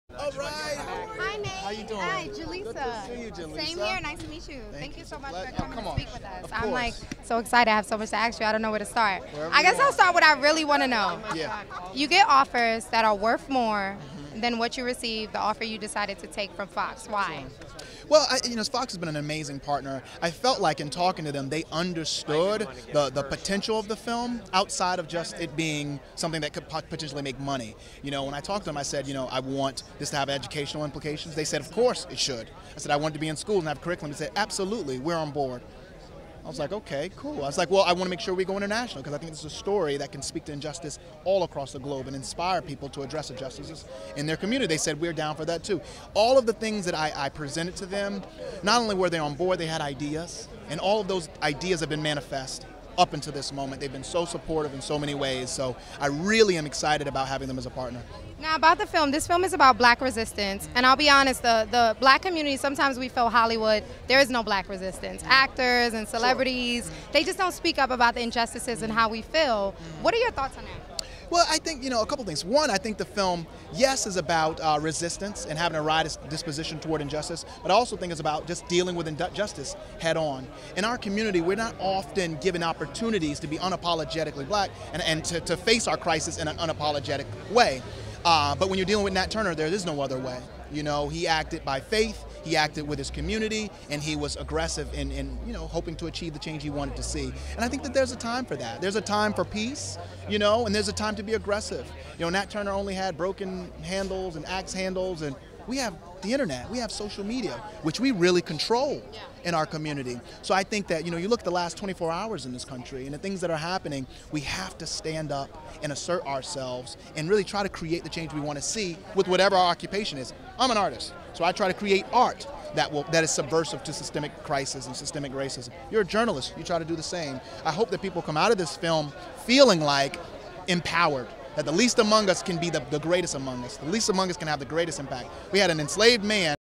Nate Parker explains why he choose Fox over Netflix for The Birth of a Nation
Nate Parker explains why he choose Fox Searchlight over Byron Allen and Netflix for The Birth of a Nation at Sundance.

Jaleesa Lashay interviews Nate Parker at The Birth of a Nation Premiere.